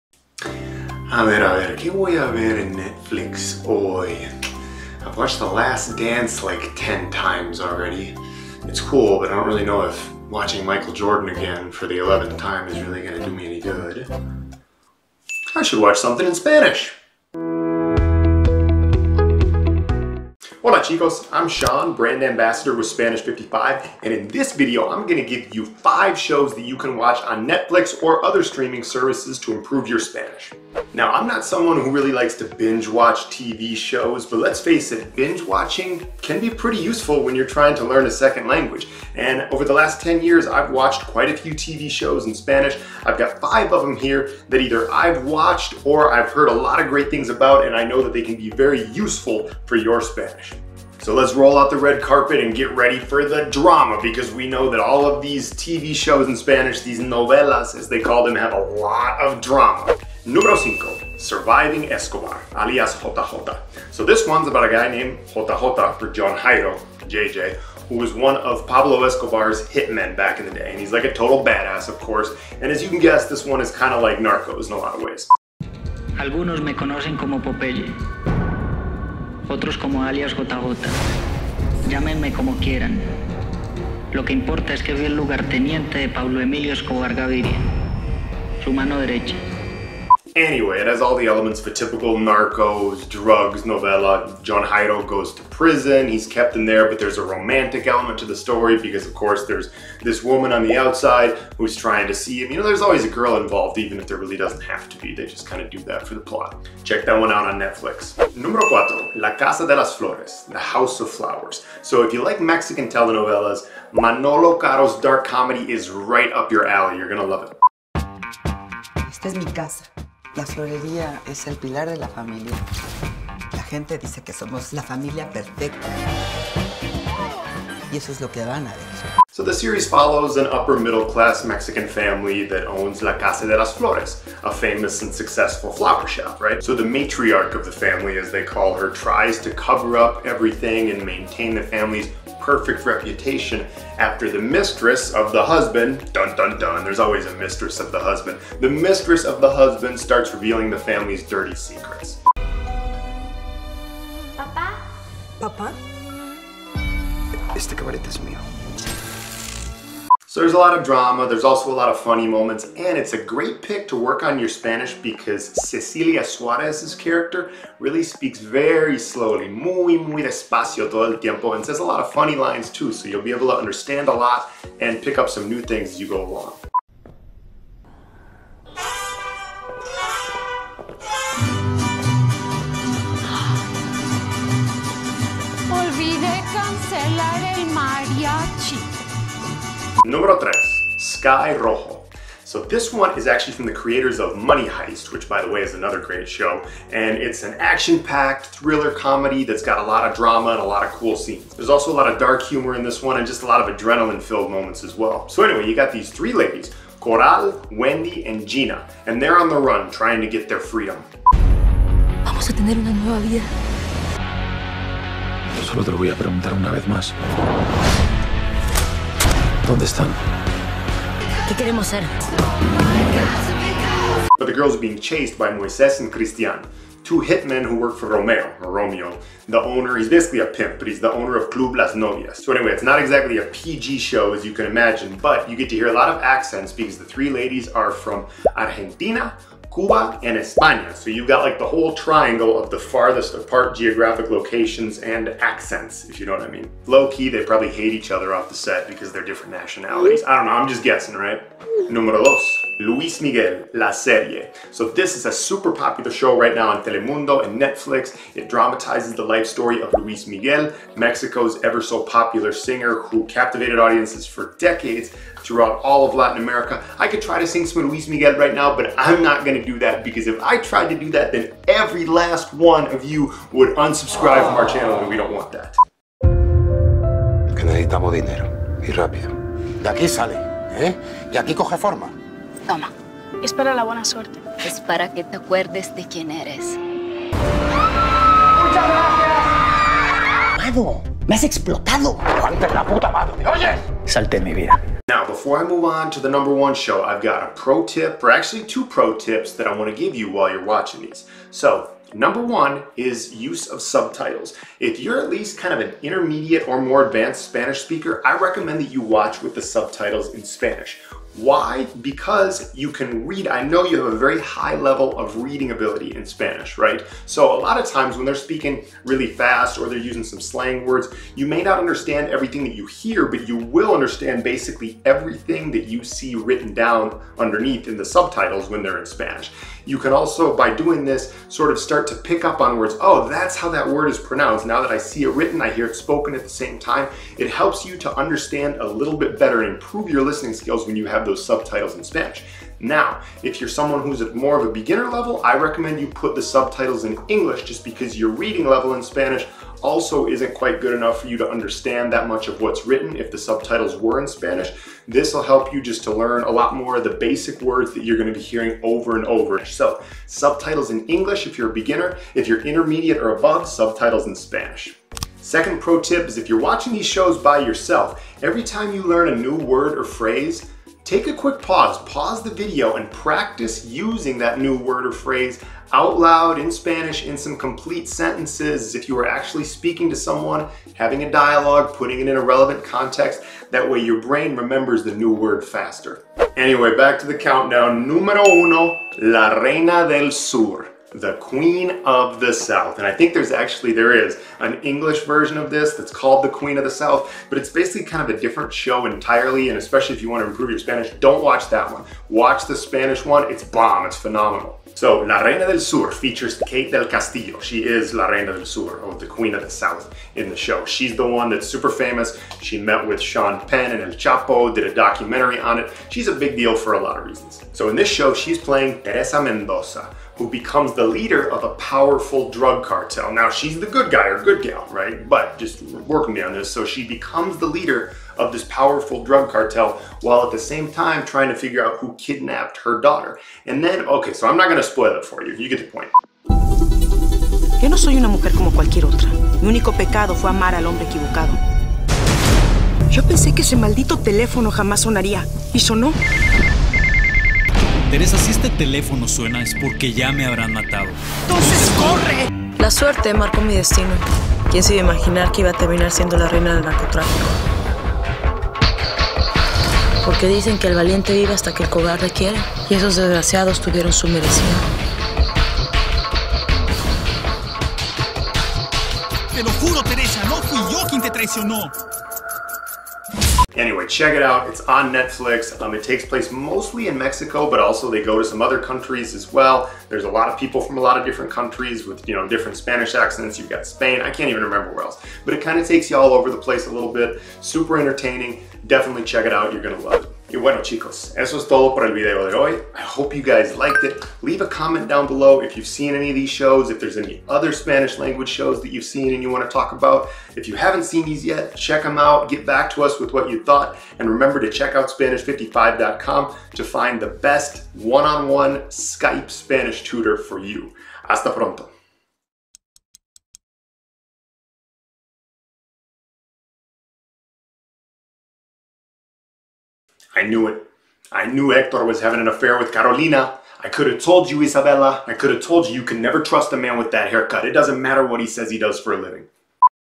Top 5 Dramatic Netflix Series to Learn Spanish
So, yeah, we've all been doing a little more binge-watching lately. So we thought it might be a great time to take a look at the Top 5 Netflix Spanish Series you can watch right now.

In this video, Brand Ambassador Sean gives a list of 5 series that go from the dramatic, to dark comedies and a dash of thriller. Because, honestly Spanish-speaking shows are ALL about the drama! *screams in spanish*

These series are great for Spanish language learners, but they’re also just totally bingeable shows, regardless of whether or not you’re brushing up your español.

So if you're looking for something a little more immersive, and a little more dramatic, but still want to learn Spanish, these five Spanish-language series are perfect.

👉 Are you looking to speak Spanish confidently and fluently?

💬 Questions? Chat LIVE with a Spanish55 team member

♥️ FOLLOW Spanish55!

CHAPTERS:

0:00 - Cold Opening
0:20 - Intro
1:00 - Show #5
1:48 - Show #4
3:10 - Show #3
4:11 - Show #2
4:55 - Pro Tips
6:26 - Show #1
8:11 - Final Thoughts